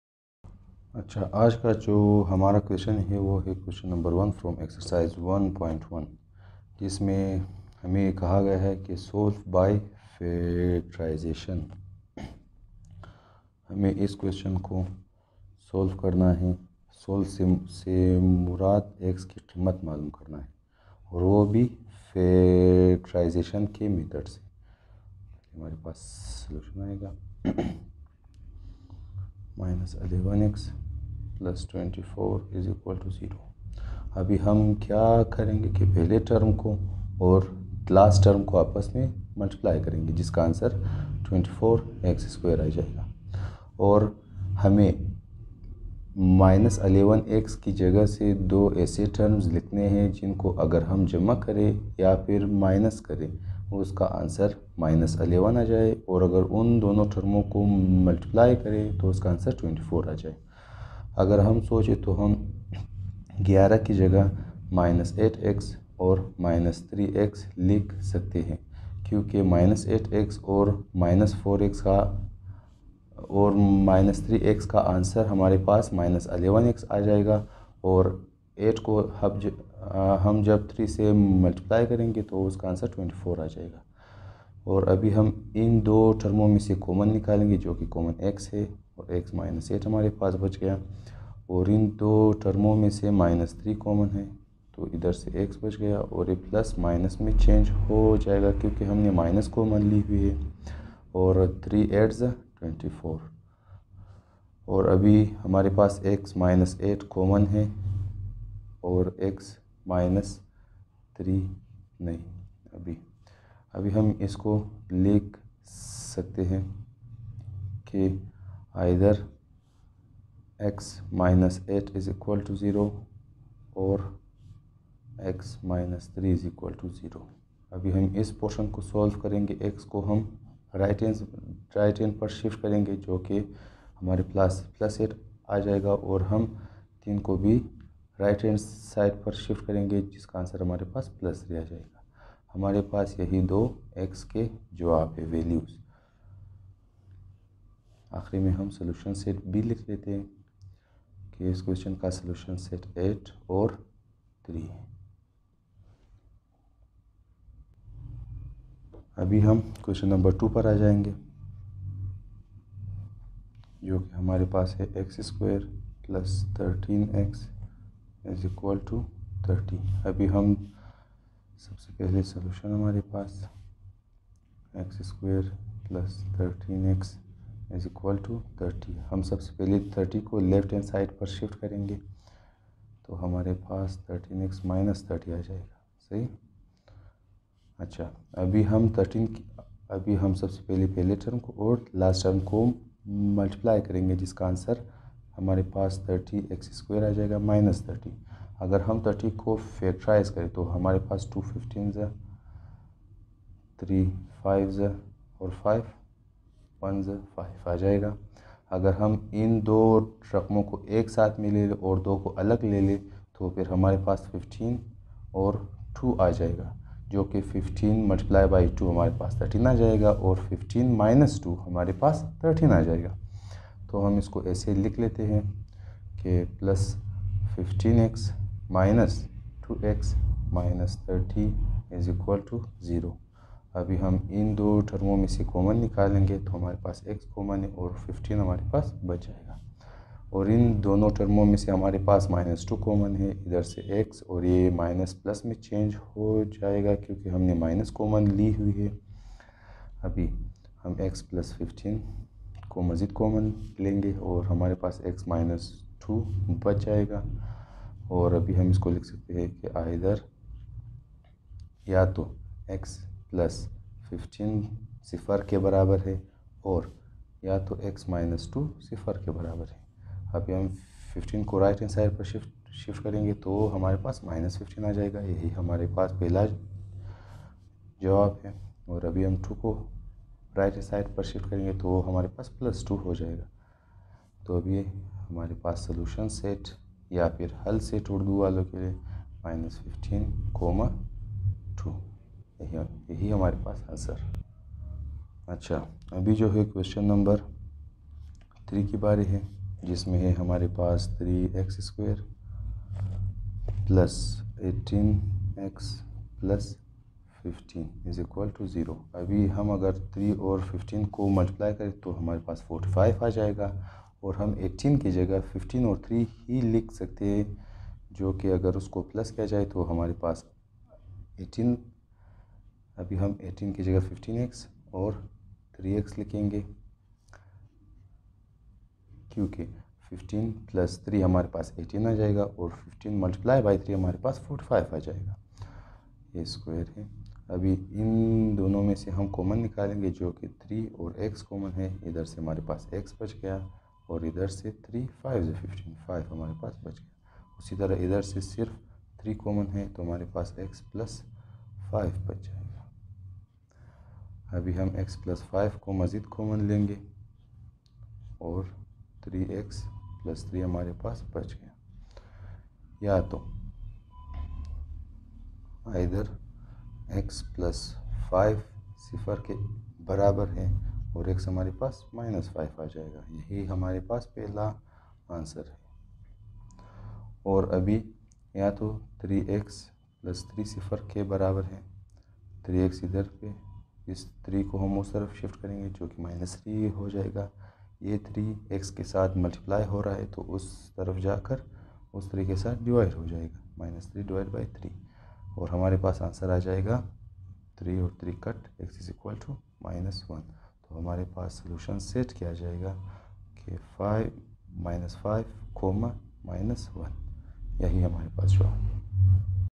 10th Class Math Exercise 1.1 Questions 1,2 and 3 | Solve by factorization | class 10 maths ex1.1 q1
10th Class Math Exercise 1.1  Questions 1,2 and 3 | Solve by factorization | class 10 maths ex1.1 q1
Solve by Factorization.
بذریعہ تجزی حل کریں۔
 Balochistan Board
Balochistan Textbook